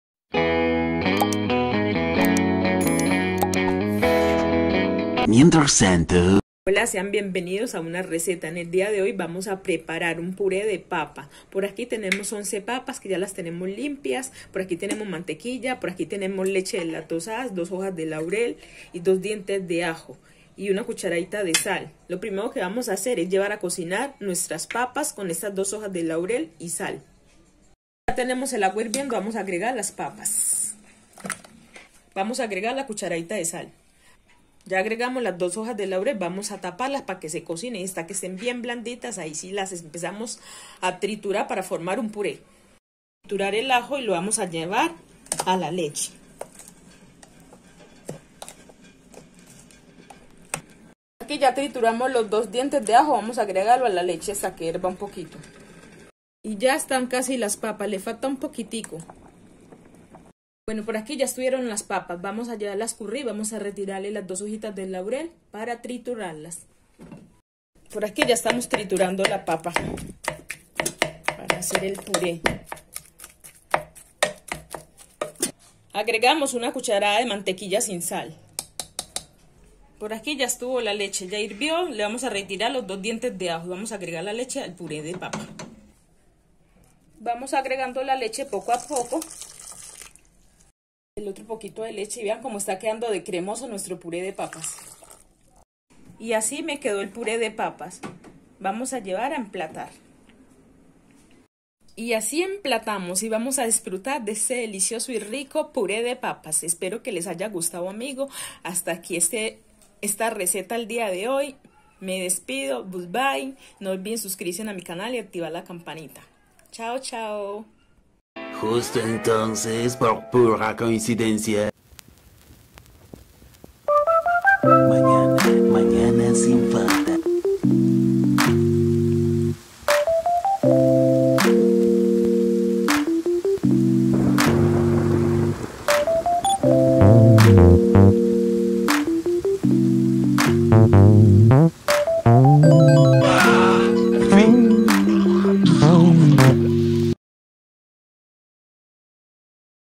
Puré de papa fácil y rápido 🍽️🤤👌😋🥔🥔🥔👩🏽‍🍳
INGREDIENTES:
11 PAPAS NORMALES
MANTEQUILLA
2 HOJAS DE LAUREL
2 DIENTES DE AJO
UN POSILLO DE LECHE DESLACTOSADA
UNA CUCHARADITA DE SAL